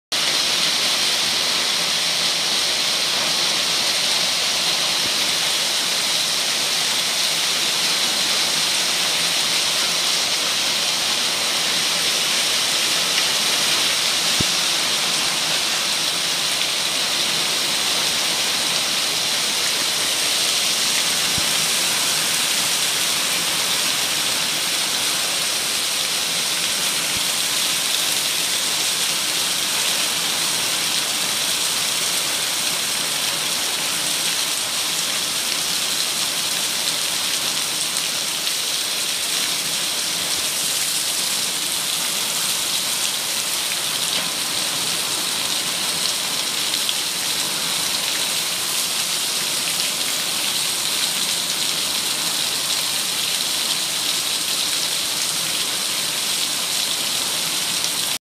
Crazy weather today in Norway!
Today it was raining so much  with ice cubes that if I went out more earlier I would have had a real headache ! So I was lucky !